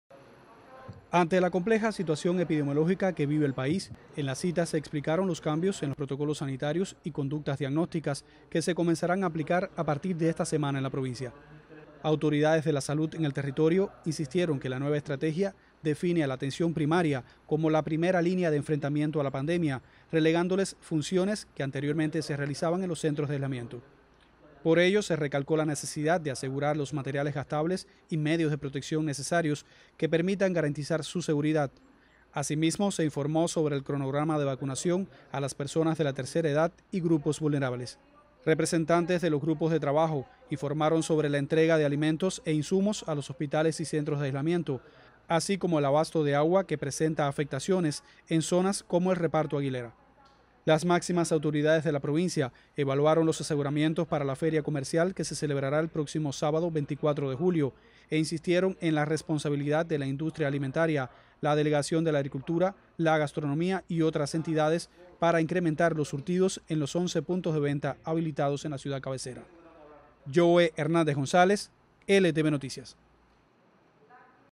Analizan en LasTunas nuevos protocolos sanitarios y aseguramientos para enfrentar la Covid_19
Un análisis sobre la situación epidemiológica y los aseguramientos para garantizar las acciones de enfrentamiento a la pandemia se realizó durante la reunión de servicios básicos en la sede provincial del Partido. Detalles en el siguiente reporte de Yoe Hernández González.